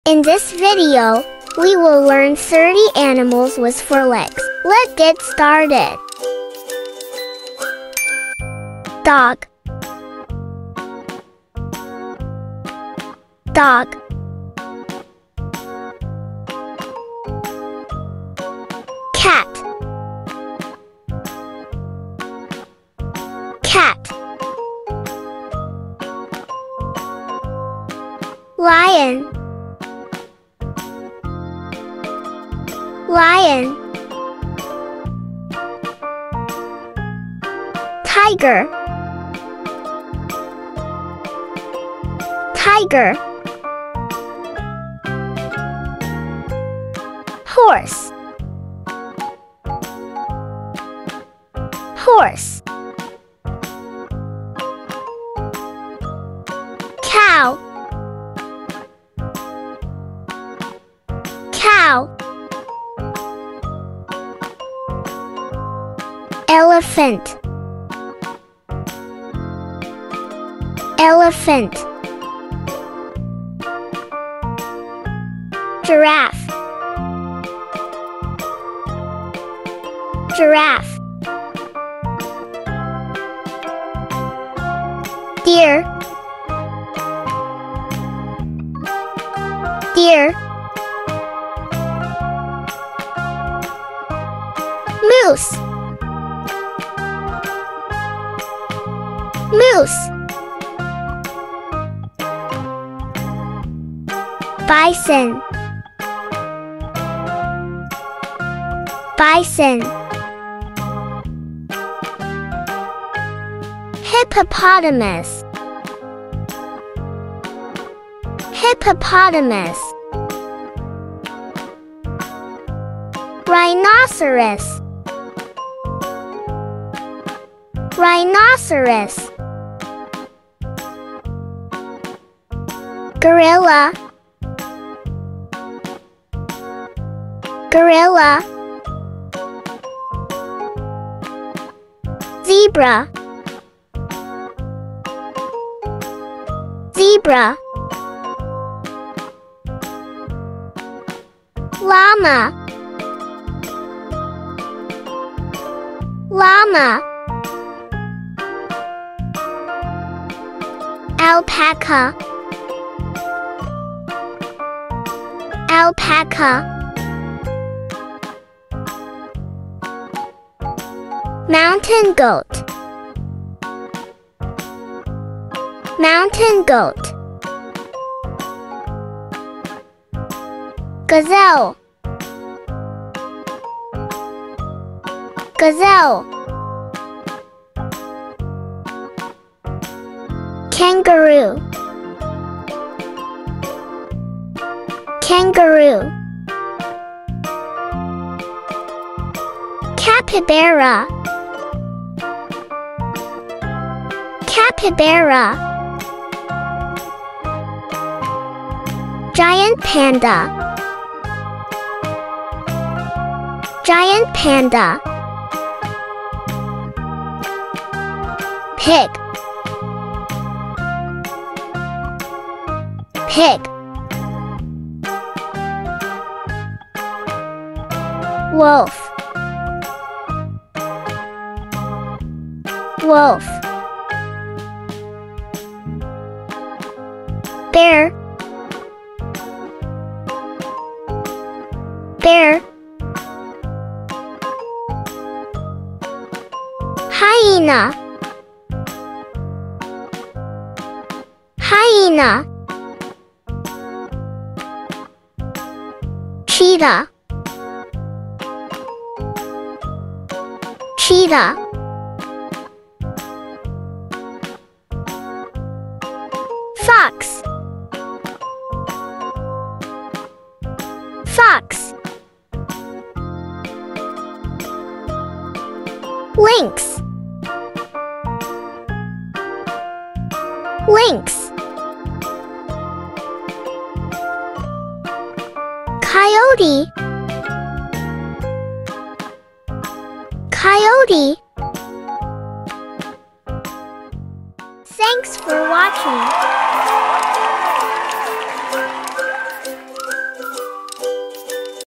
30 Animals with Four Legs for Kids to Learn - Educational Video
We bring you HD quality videos from 30 Animals with Four Legs to learn their names. Learning the 30 Animals with Four Legs Names correctly is very important for toddler, pre school or elementary school children.

List of 30 Animals with Four Legs Names in the World :
Dog
Cat
Lion
Tiger
Horse
Cow
Elephant
Giraffe
Deer
Moose
Bison
Hippopotamus
Rhinoceros
Gorilla
Zebra
Llama
Alpaca
Mountain goat
Gazelle
Kangaroo
Capybara
Giant Panda
Pig
Wolf
Bear
Hyena
Cheetah
Fox
Lynx
Coyote